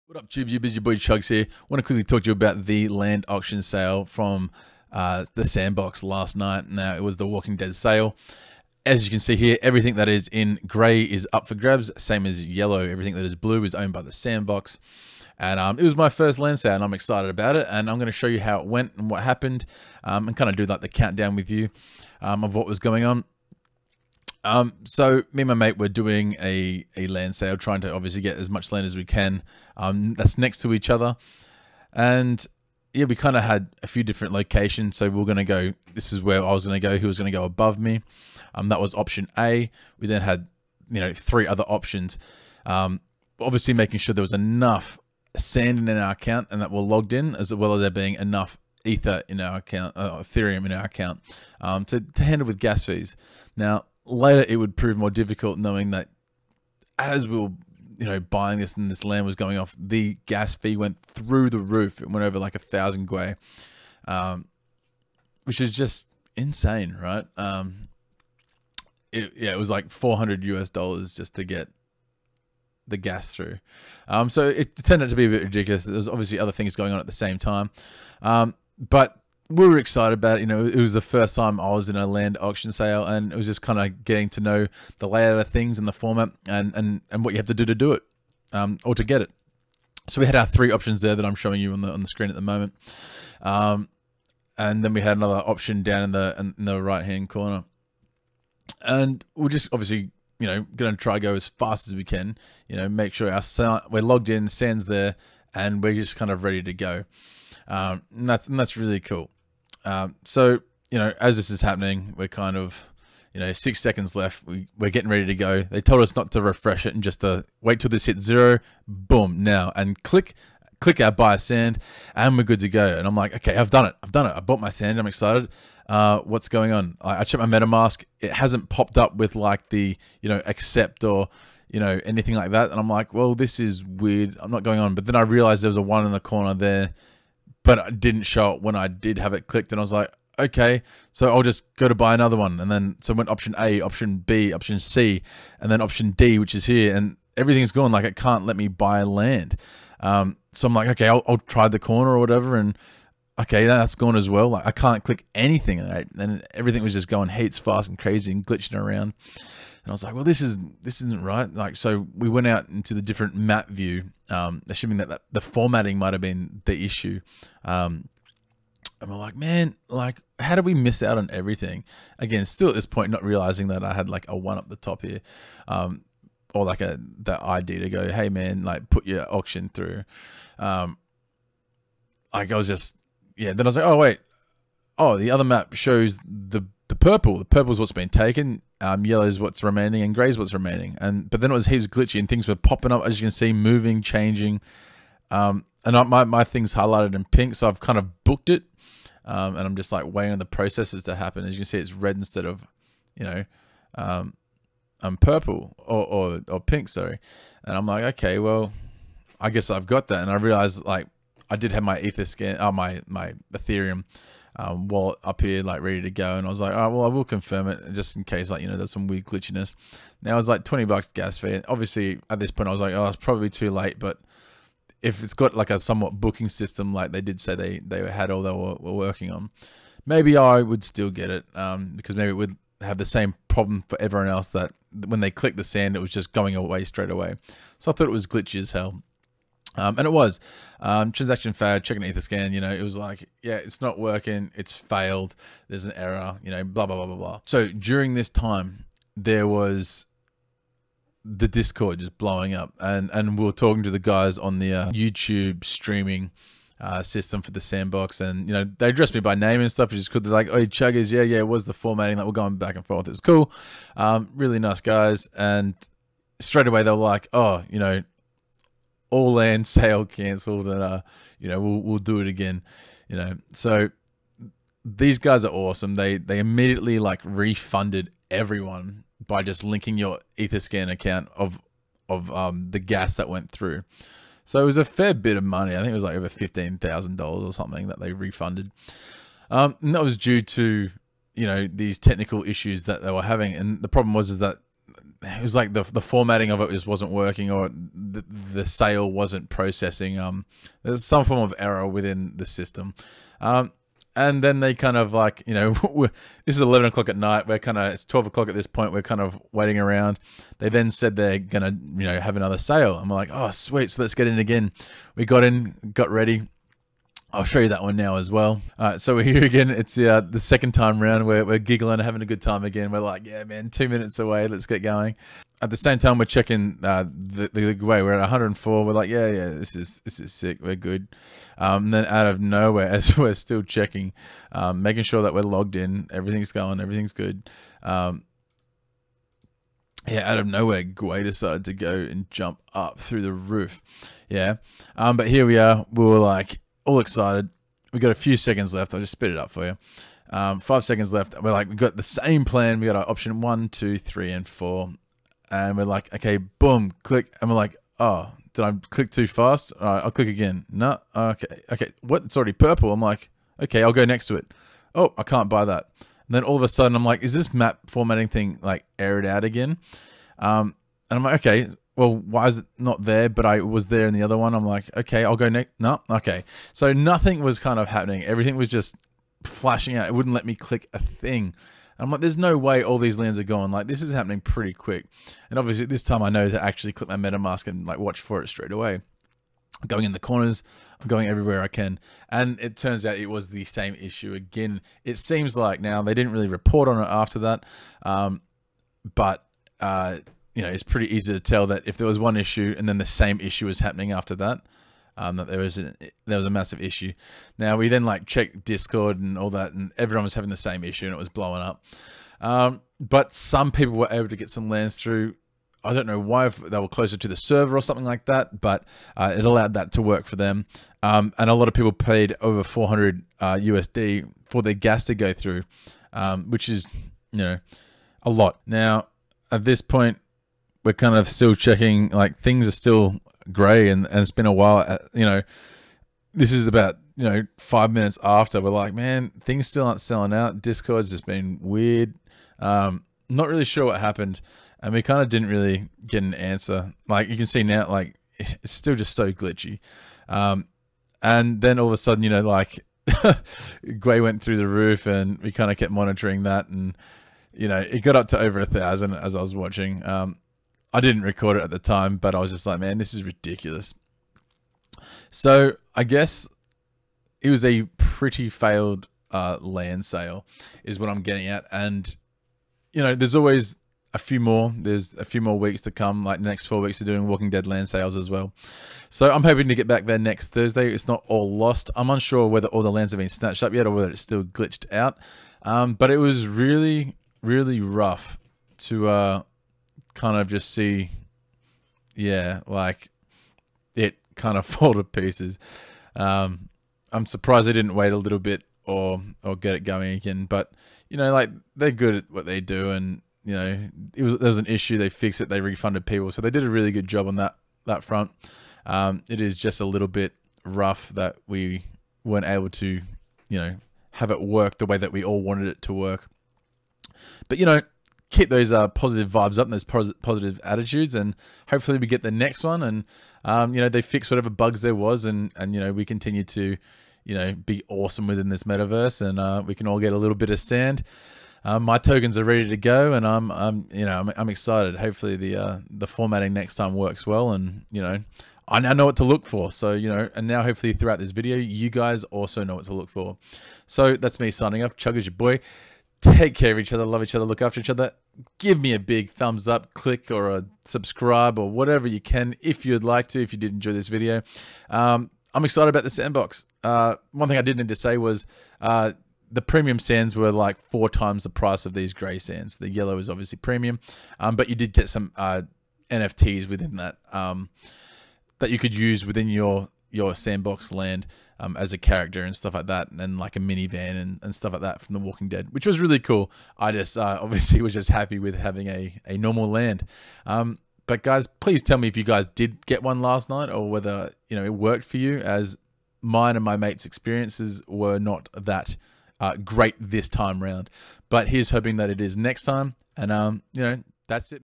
The Sandbox land sale kerfuffle. Trying to buy land failed....what went wrong????
hi guys, last night me and a friend tried to buy some sandbox land. VERY exciting times, however it did not go to plan. Luckily the sandbox crew refunded WASTED eth gas, and a second auction took place, unfortunately, same issues arose and I MISSED OUT on land.

BNB/BSC:0xe1Cc9fCa908aAAC30616826D5a320e5Bc5C9223b
ETH/ERC20:0xe1Cc9fCa908aAAC30616826D5a320e5Bc5C9223b